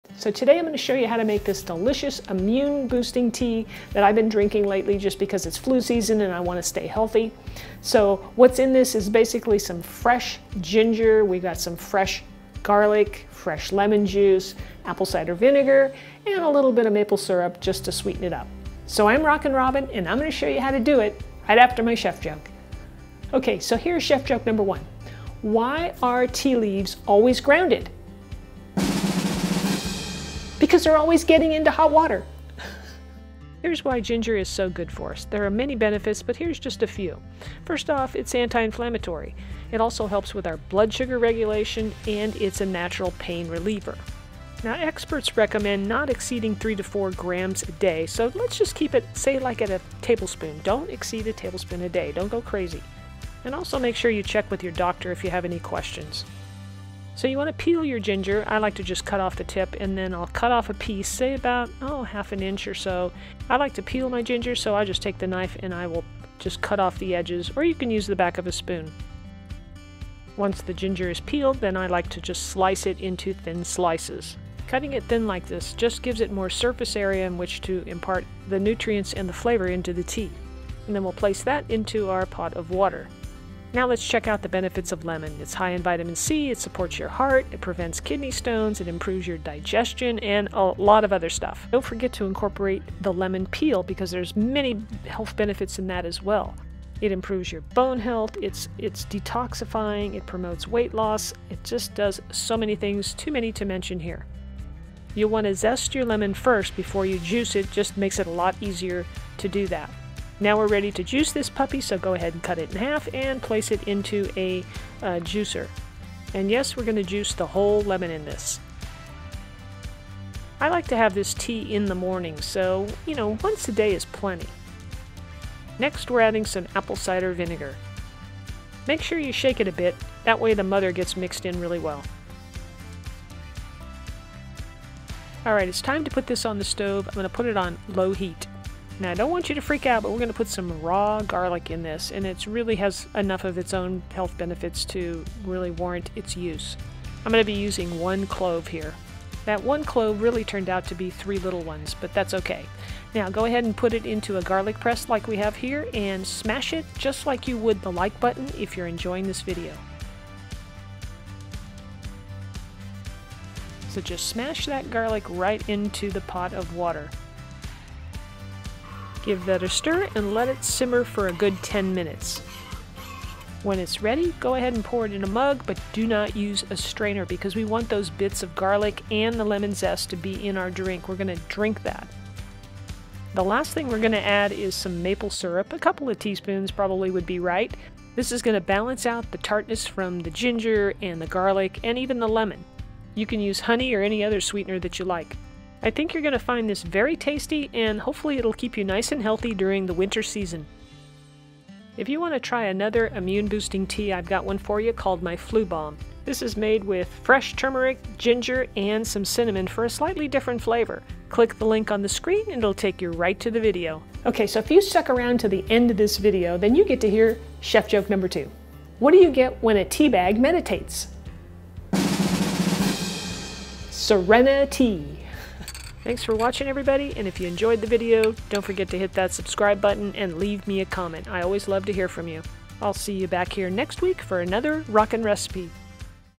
Ginger Tea Boosts Your Immune System - Here's Why You Need To Add It To Your Diet
In this video, we're going to discuss the amazing benefits of ginger tea. Not only is ginger a great way to boost your immune system, but it's also delicious and easy to make.

If you're looking for a drink that will help you feel healthier and more energetic, ginger tea is a great option. This tea is especially good for people who suffer from colds or other types of infections. By drinking ginger tea, you'll not only boost your immune system, but you'll also enjoy the delicious taste!
This ginger, lemon, garlic tea has many benefits and will help keep you healthy during the flu and cold season. It's easy and quick to make. In fact you can double the recipe for those with a busy schedule so you don't have to make it everyday.

Time Stamps
00:00 Intro
00:23 Chef Joke
00:35 Benefits of ginger
00:56 preparing the ginger
01:24 benefits of lemon
01:31 preparing the lemon
02:06 adding the apple cider vinegar
02:20 benefits of garlic
02:26 prepping the garlic
02:50 simmer the tea
02:56 serve up the tea
03:03 sweeten tea
03:22 another delicious immune boosting tea to try
03:34 Chef joke #2
03:46 Outro

▶︎ Tortilla Warmer

▶︎ Wooden Spoons

▶︎ Vitamix 5200

▶︎ Cooling Racks

▶︎ Arrowroot Powder

▶︎ Taco Holders

Ginger has anti inflammatory properties, helps relieve nausea, helps with blood sugar regulation, It's loaded with antioxidants which helps protect against disease and it is a  natural pain reliever to name a few.

Lemon is high in vitamin C, supports heart health, prevents kidney stones, and improves digestive health.

Garlic is anti inflammatory,  anti bacterial, helps boost your immune system, and it might even prevent Alzheimer's and dementia.

Ginger Lemon Garlic Tea

Ingredients:
½ inch of fresh ginger, peeled and thinly sliced
1 whole organic lemon, zested, juiced
1 Tbsp. apple cider vinegar
1 -2 cloves garlic, minced
1 - 2 tsp. Maple syrup
12 oz of water

Directions:

Put the water and all the ingredients except the maple syrup into a saucepan over low heat. Bring to a simmer over medium heat and them simmer for 10 minutes.

Pour tea into a mug and be sure to not strain it. We want to drink the garlic and lemon zest. Add the maple syrup to your desired sweetness and enjoy!

Substitutions:
You can use any sweetener you like, honey, agave syrup or even sugar (white, coconut, or brown). If you use honey, add it after the tea cools a bit so as to not destroy the benefits of the honey.

Rockin Robin
P.S. Please help me spread the word about my channel.

~ Rockin Robin